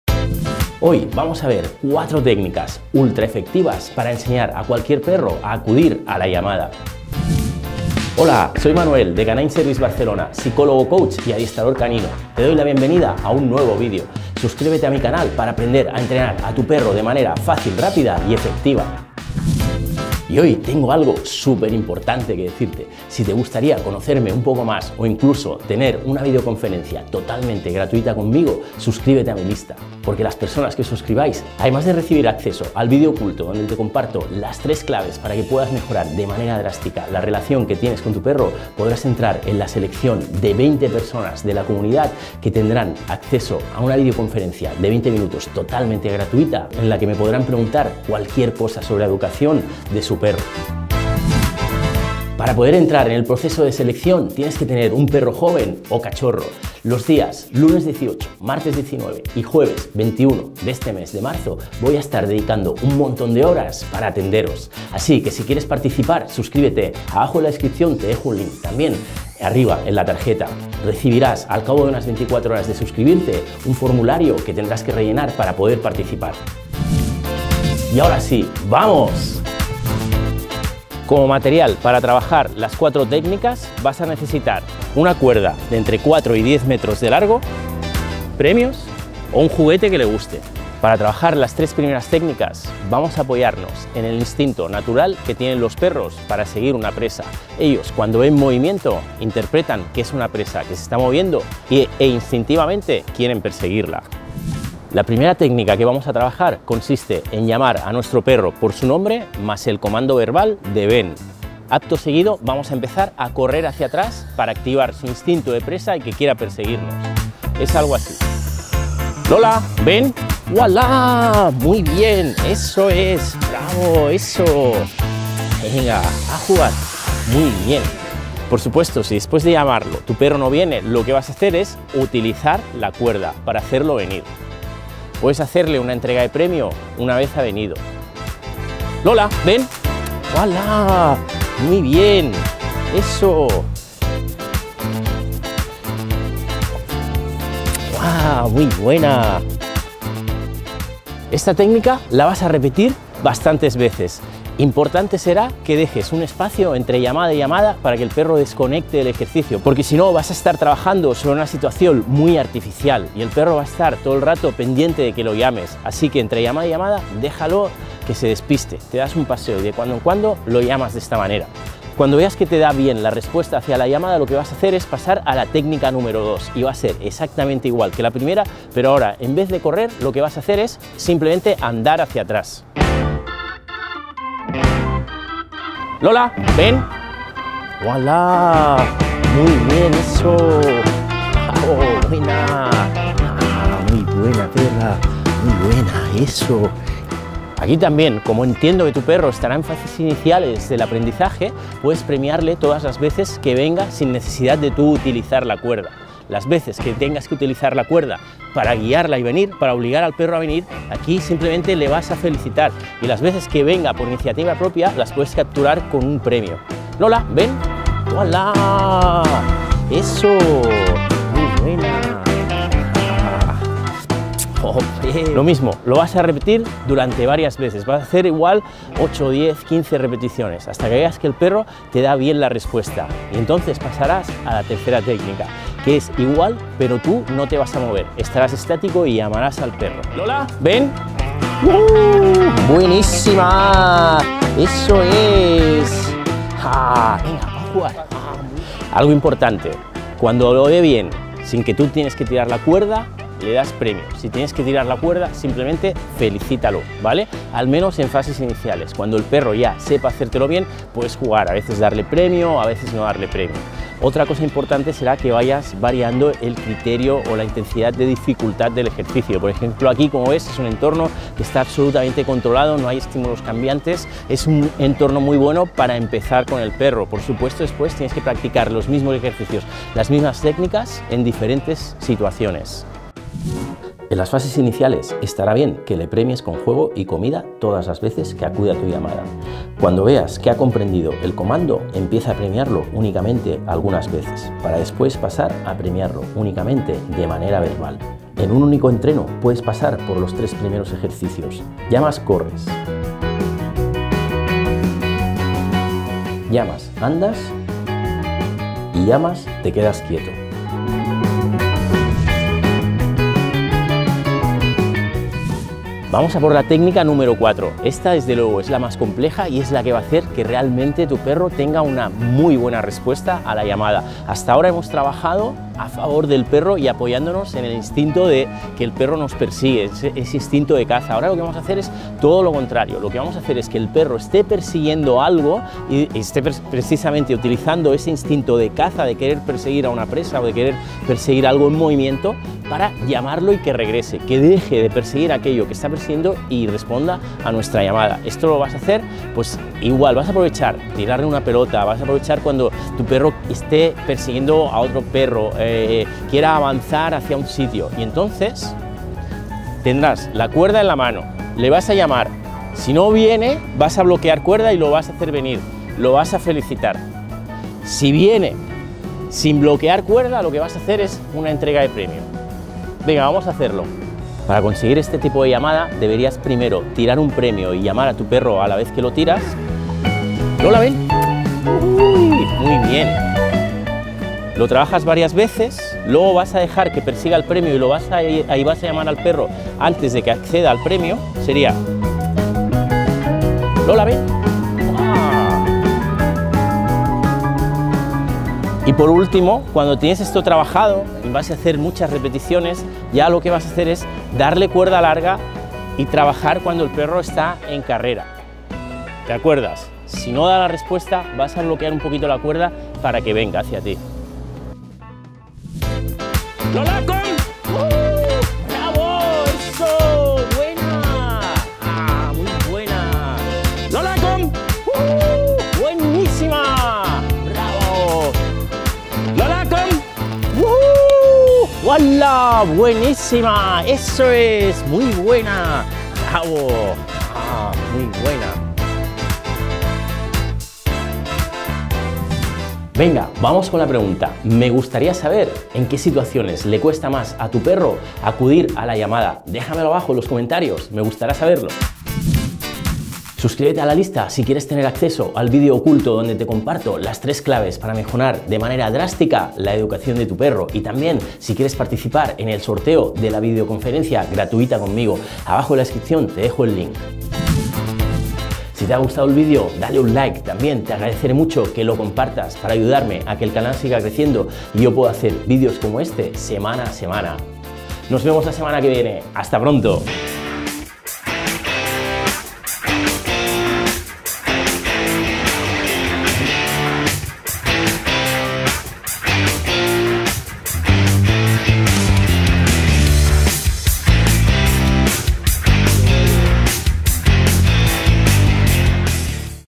PRIMERA ORDEN que debes ENSEÑAR a tu PERRO VENIR CUANDO lo LLAMAS
La orden de venir a la llamada o llamado es una de las más importantes. Hoy te explico 4 técnicas de adiestramiento canino para que puedas entrenar a tu perro a obedecer y acudir a tu llamada.

☝¿Quieres descubrir cuál es la causa real de sus problemas y por qué no puedes controlar sus conductas? Consigue acceso al vídeo oculto donde te lo explico

      Categoría Mascotas y animales
      LicenciaLicencia Atribución de Creative Commons (reutilización permitida)